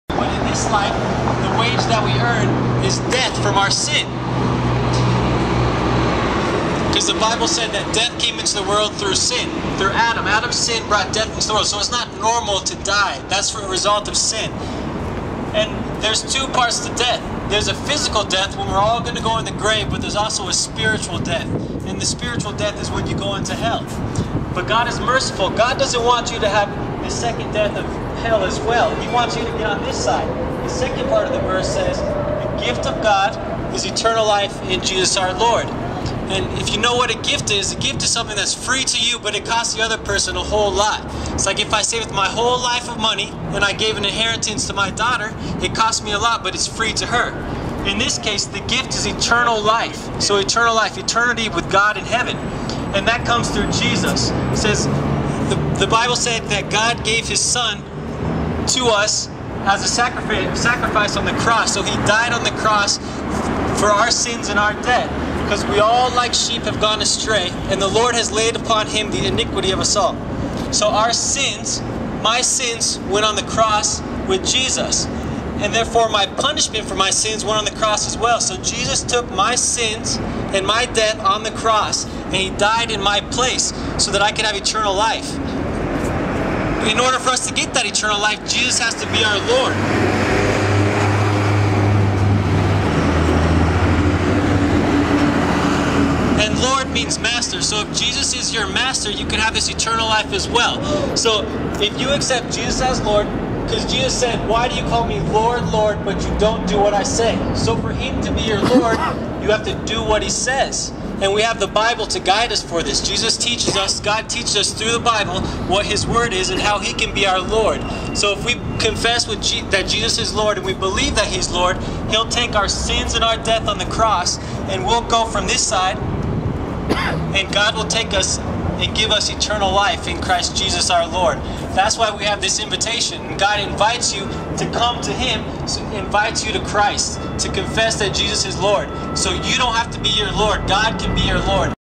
Street Preaching, Michel Okoh
Michel makes his debut street preaching at the 16th and Stout light rail station.